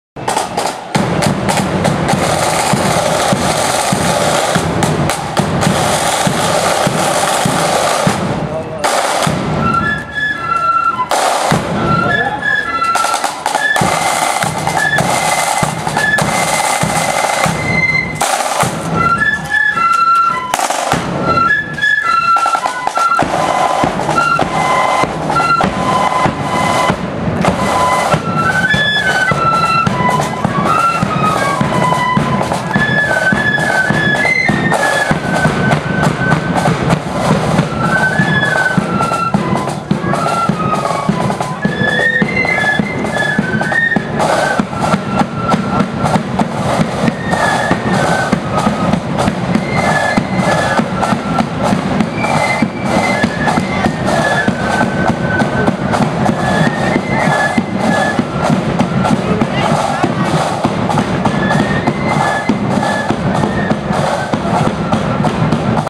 Robert Graham Mem @ Cairncastle 03/05/2014
Robert Graham Memorial On the March in Larne for Cairncastle annual band parade Saturday 3rd May 2014